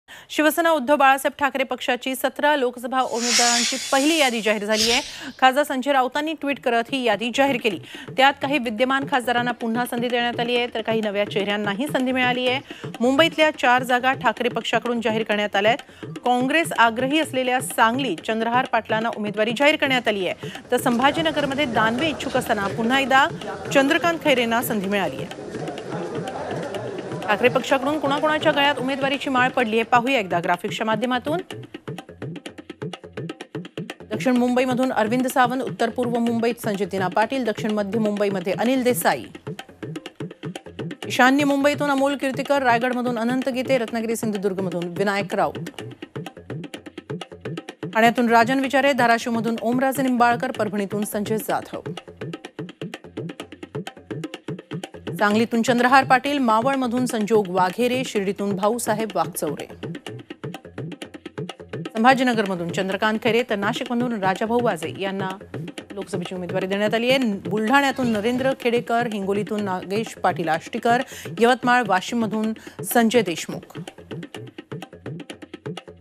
Shivena Lok Sabha List शिवसेना उद्धव बाळासाहेब ठाकरे पक्षाची 17 लोकसभा उमेदवारांची पहिली यादी जाहीर
शिवसेना उद्धव बाळासाहेब ठाकरे पक्षाची 17 लोकसभा उमेदवारांची पहिली यादी जाहीर झालीय. खासदार संजय राऊतांनी ट्विट करत यादी जाहीर केली. त्यात काही विद्यमान खासदारांना पुन्हा संधी देण्यात आली तर काही नव्या चेह-यांनाही संधी मिळालीय. मुंबईतल्या 4 जागा ठाकरे पक्षाकडून जाहीर करण्यात आल्यात. काँग्रेस आग्रही असलेल्या सांगलीत चंद्रहार पाटलांना उमेदवारी जाहीर करण्यात आलीय. तर संभाजीनगरमध्ये दानवे इच्छुक असताना पुन्हा एकदा चंद्रकांत खैरेंना संधी मिळालीय.